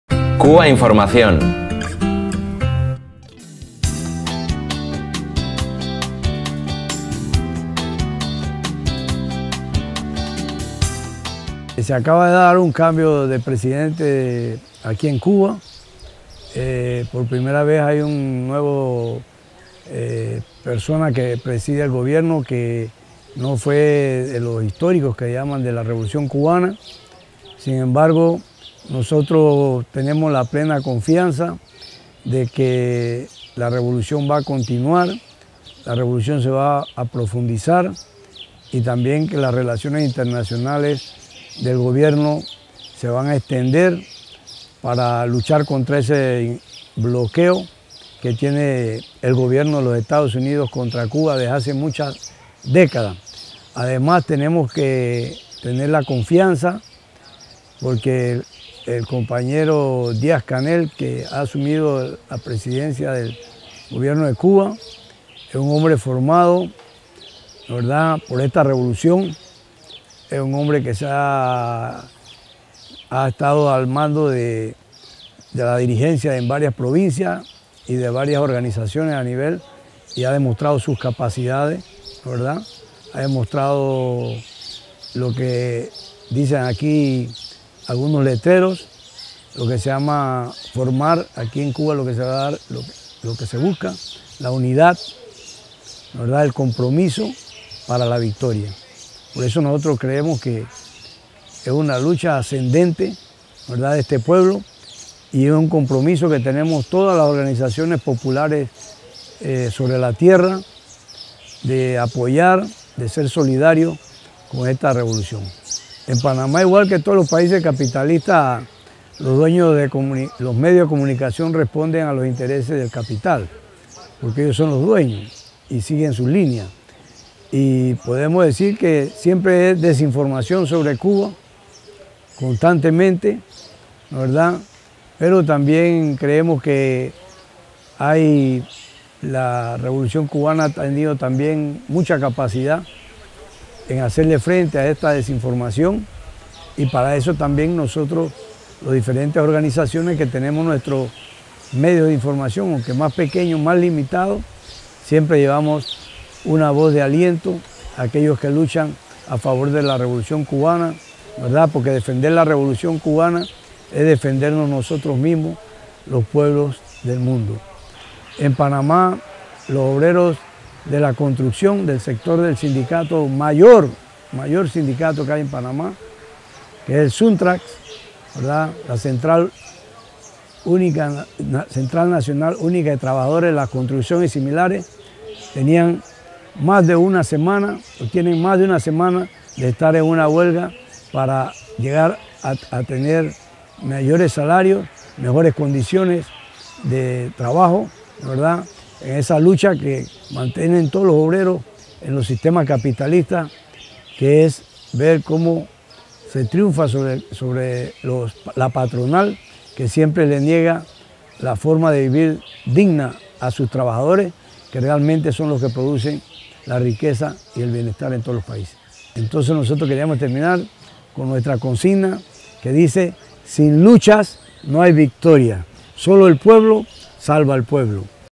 `En Cuba y en Panamá, sin lucha no hay victoria, solo el pueblo salva al pueblo´
Eric Fernández pertenece al Frente Nacional por la Defensa de los Derechos económicos y Sociales en Panamá y a la Unión Campesina Panameña. Realiza un contraste entre los derechos de los trabajadores y trabajadoras en los países capitalistas de la región y los de Cuba, y confía en el relevo gubernamental en la Isla. Fuente